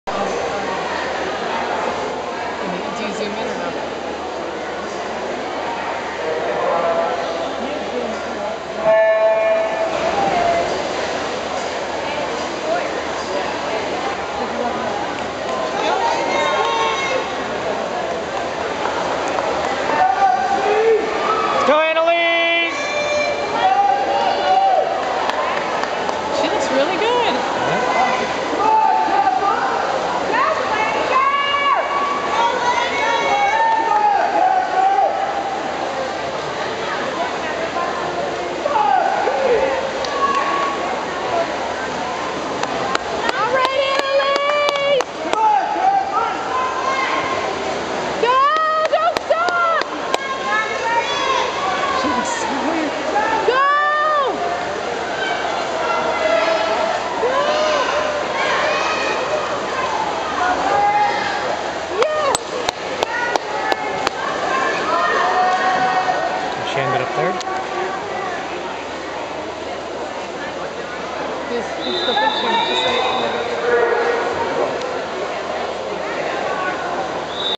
Annalise Swims the 50m Freestyle
Annalise swims the 50m freestyle.  Annalise is part of the Norwalk Zeus swim team.  This swim meet was held at Wesleyan University and the first time Annalise competed in an Olympic size pool.